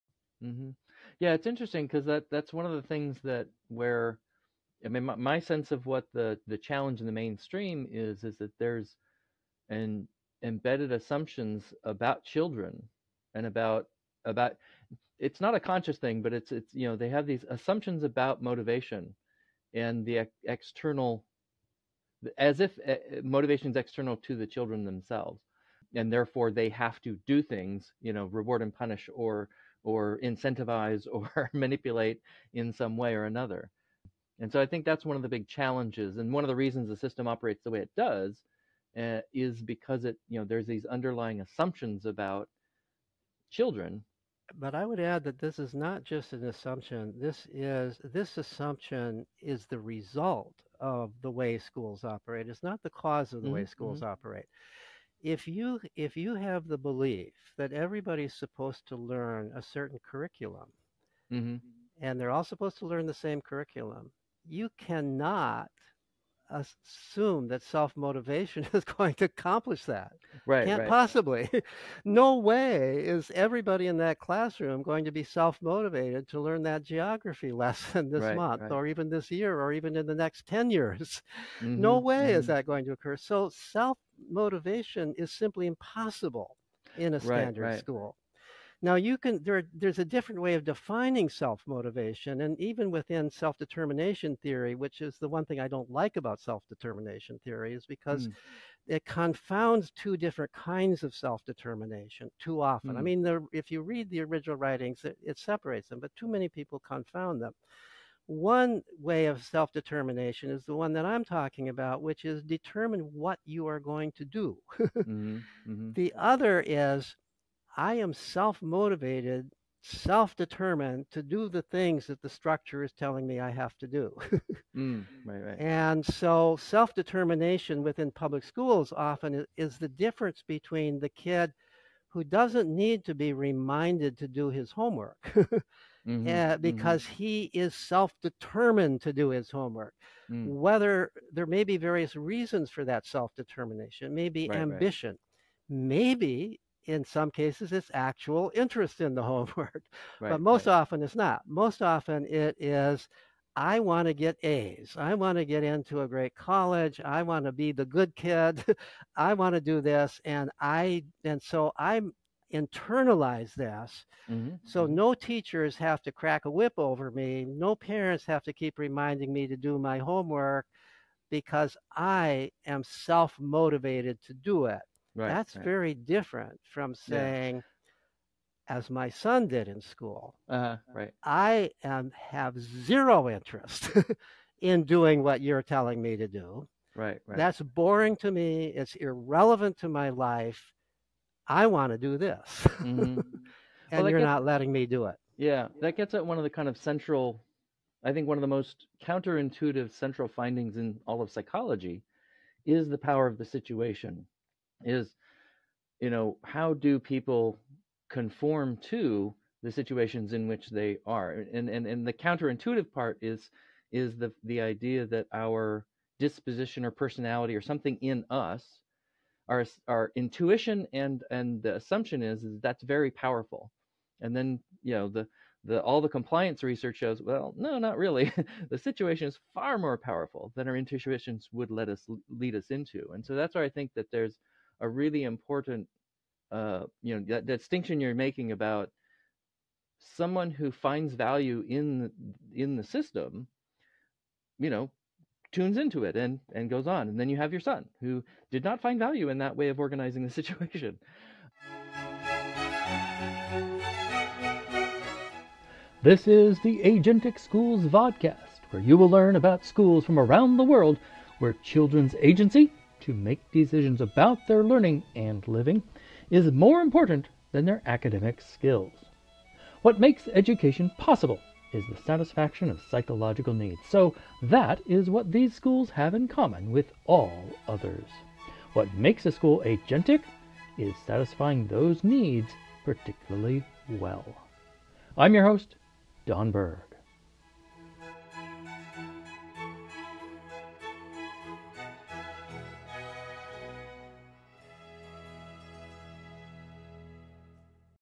Self-motivation is contradicted by the way mainstream schools are organized- Peter Gray - S2E09x9
Guest: Peter Gray, Co-founder of Let Grow and Alliance for Self-Directed Education (ASDE)